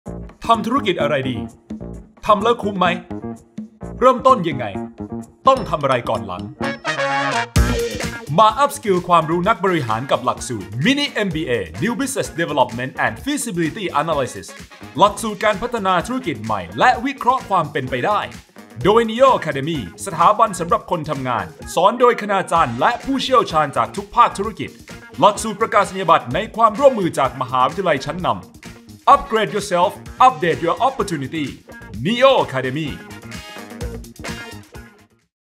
MINI MBA - New Business Development & Feasibility Analysis
ทำธุรกิจอะไรดี ทำแล้วจะดีมั้ย
ทำธุรกิจต้องเริ่มตรงไหน ทำอะไรก่อน-หลัง

มา Upskills ความรู้ด้านบริหารเพื่อสร้างธุรกิจใหม่ที่เป็นไปได้
กับหลักสูตร Mini MBA - New Business Development & Feasibility Analysis
หลักสูตรการพัฒนาธุรกิจใหม่และวิเคราะห์ความเป็นไปได้
เรียนจบรับประกาศนียบัตร Mini MBA
จากวิทยาลัยการจัดการ มหาวิทยาลัยมหิดล

ทำธุรกิจใหม่เริ่มต้นยังไง 00:05
MINI MBA - New Business Development & Feasibility Analysis 00:11
รับ Certificate of Completion 00:26